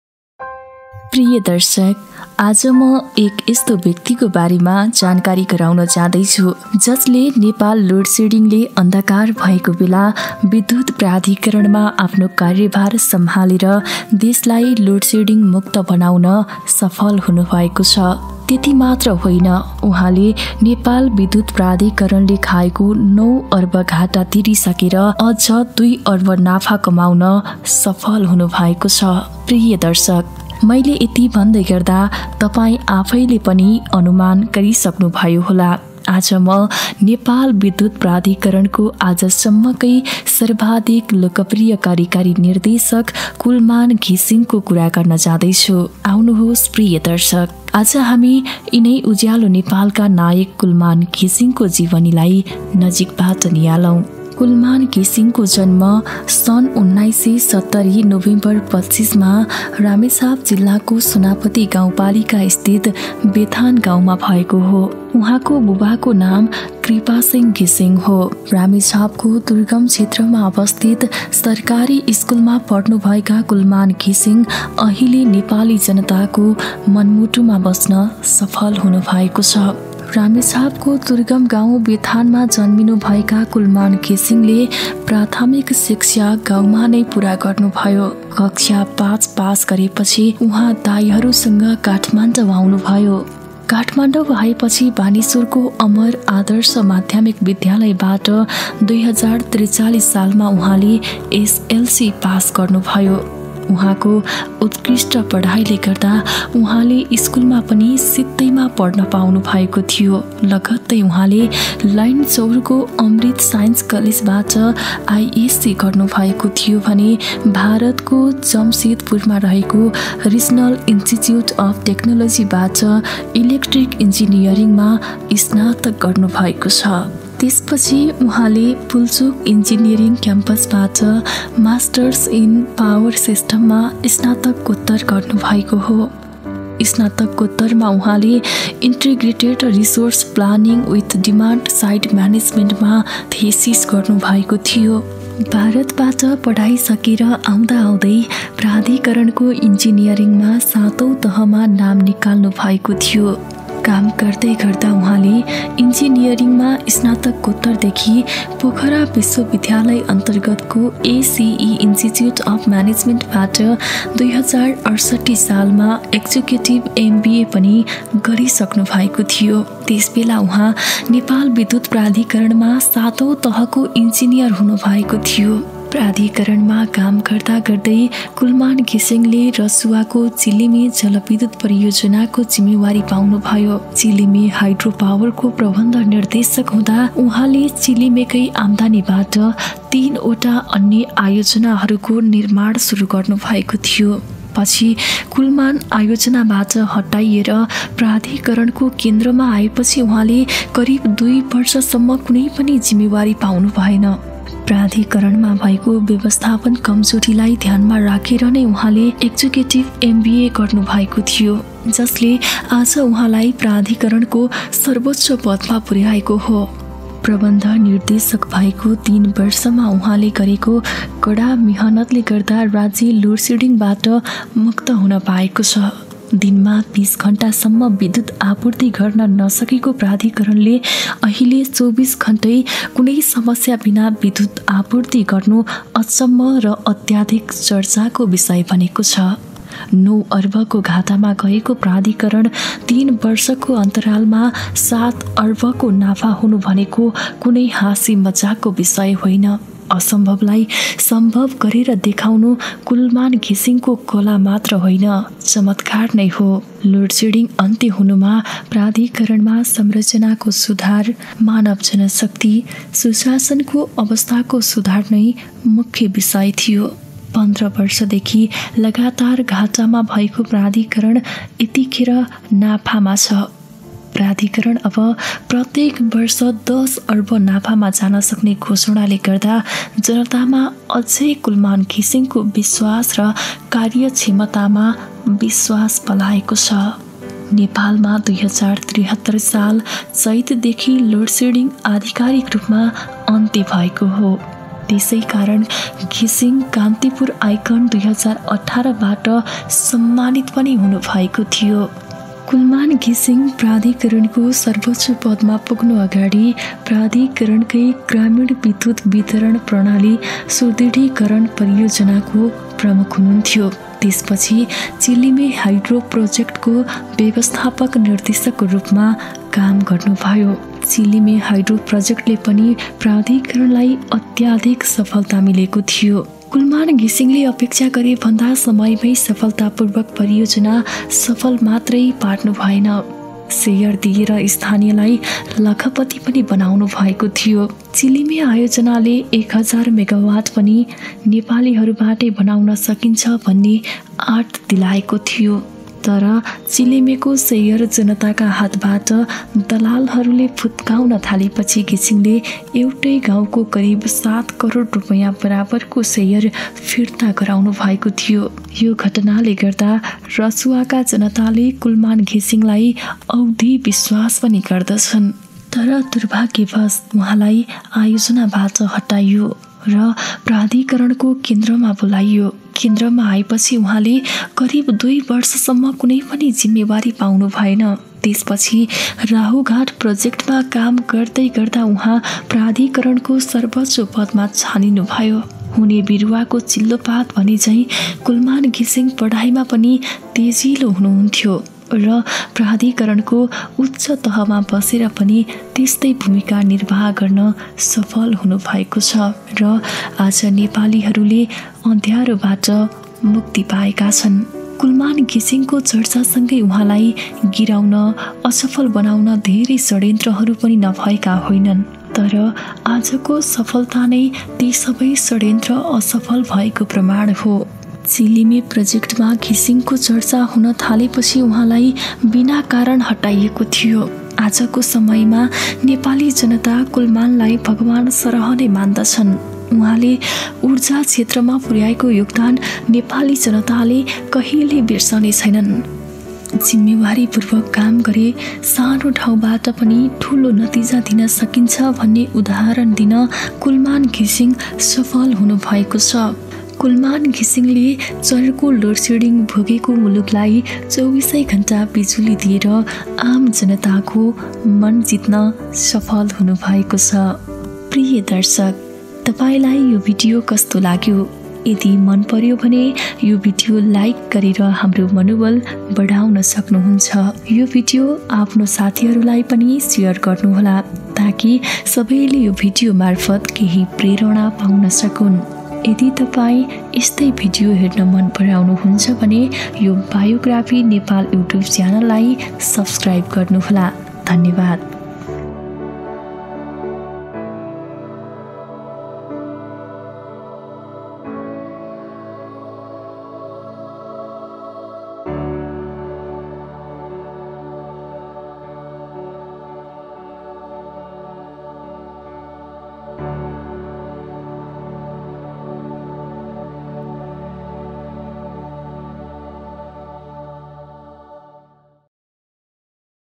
Kulman Ghising Biography | Success Story | Executive Director of Nepal Electricity Authority
This video is about Kulman Ghising Success Story and Biography In Nepali.This Motivational and inspirational Animated Video is about Managing Director of Nepal Electricity Authority.

Kul Man Ghising (Nepali: कुलमान घिसिङ) is the managing director of Nepal Electricity Authority (NEA). He is known for solving the load shedding problem in Nepal, whereby, for decades, the country had power outages of up to 18 hours daily, within two months of taking office.

Ghising was appointed the mananging director of NEA on 14 September 2016 by a cabinet decision. He had been associated with NEA for over two decades prior to the appointment. At the time of the appointment, he was serving as the project chief of Rahughat Hydroelectricity Project, having been managing director of the Chilime Hydropower Company before that.

About This Video :
1) kulman ghising biography
2) kulman ghising latest interview
3) kulman ghising latest news
4) kulman ghising protest
5) kulman ghising ko jibani